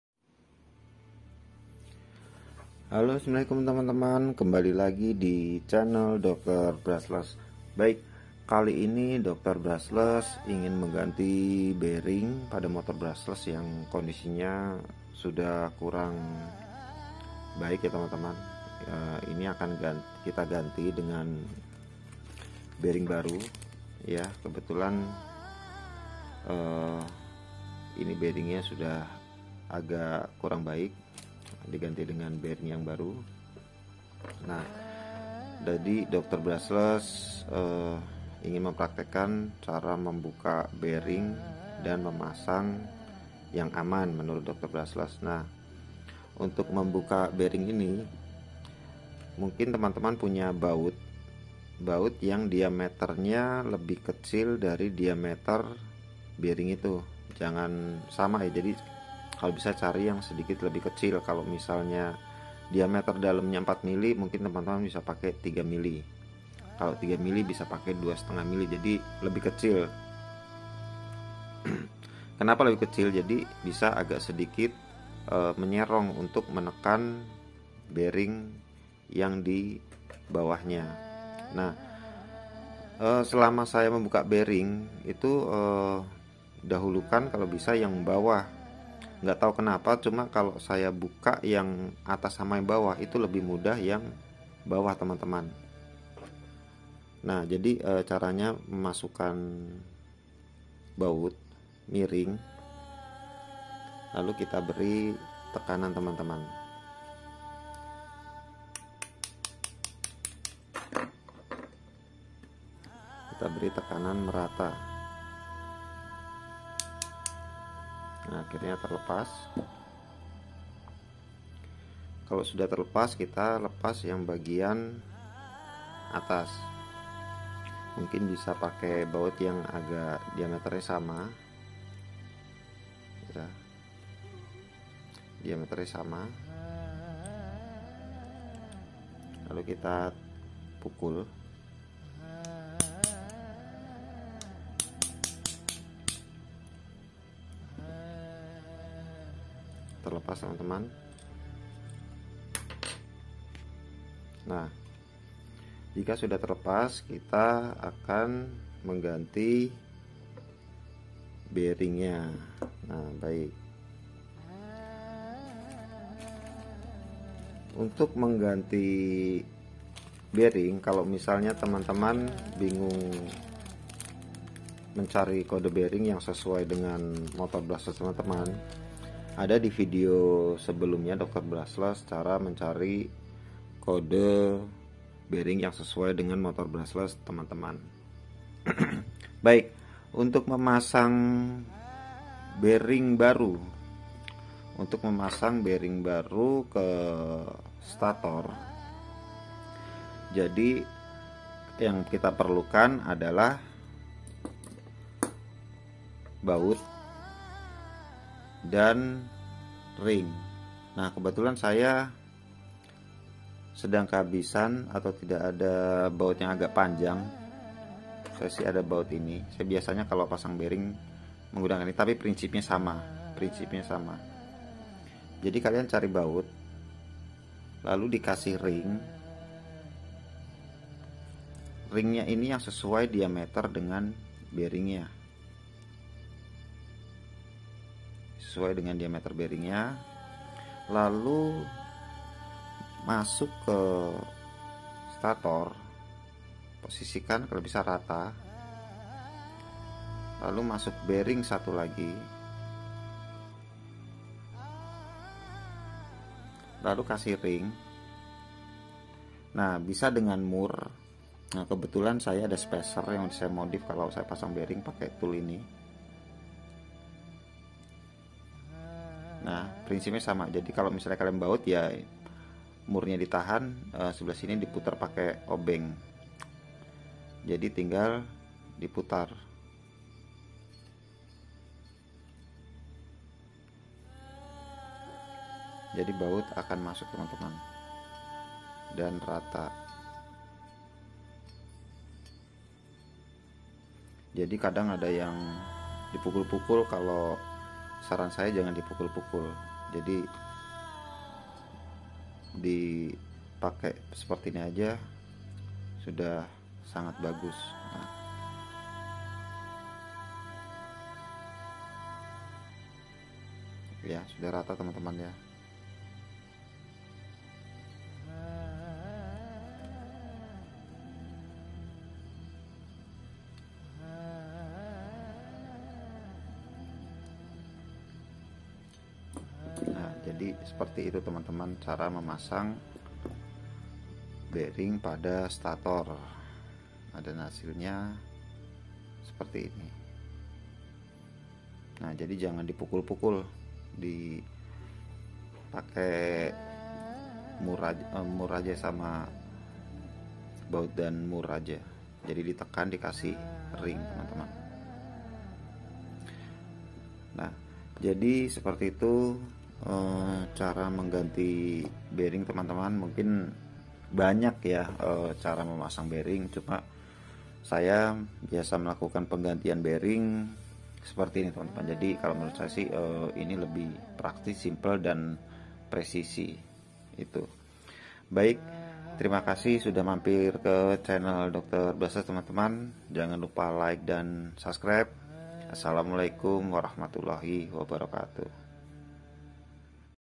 Motor brushless kasar , cara mengganti bearing pada motor brushless
dari sekian banyak metode dan cara dalam mengganti bearing ini adalah salah satu cara termudah dalam membuka dan memasang bearing tanpa melukai stator